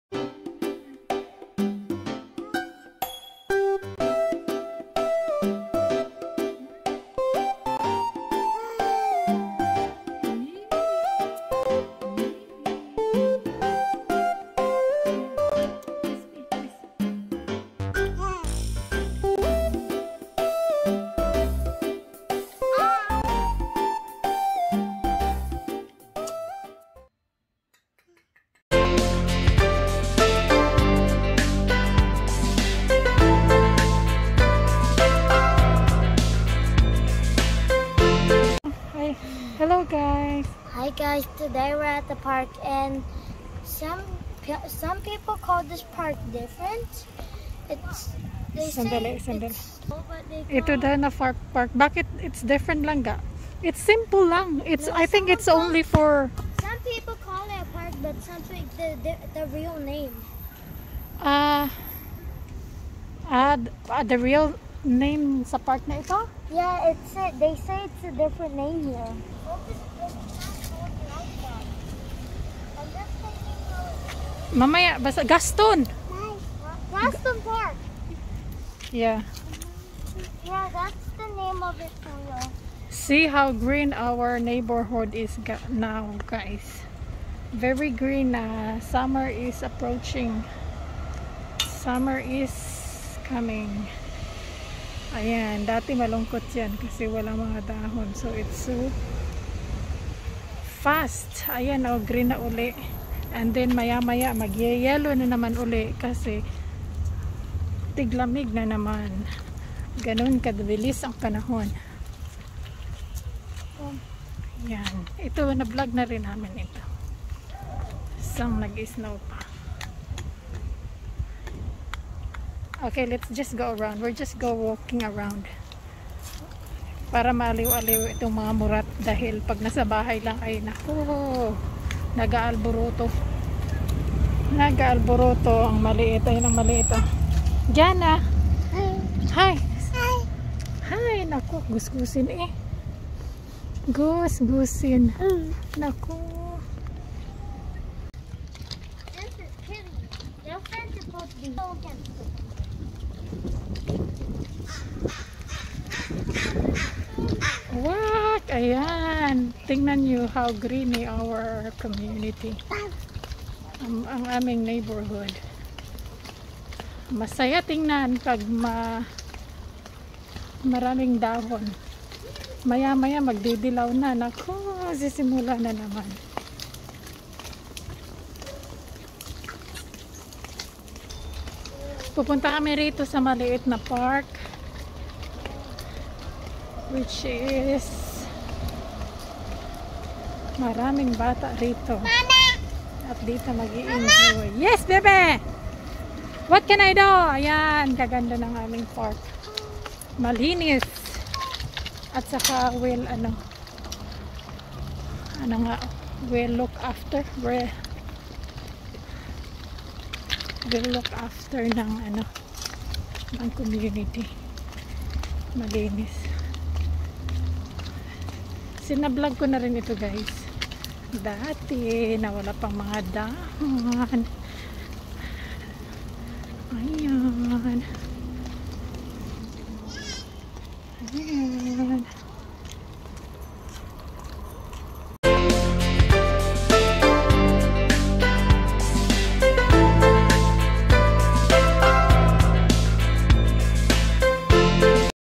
Walking around our neighborhood and getting some fresh air || Collingwood Neighborhood
Collingwood Neighborhood  Community.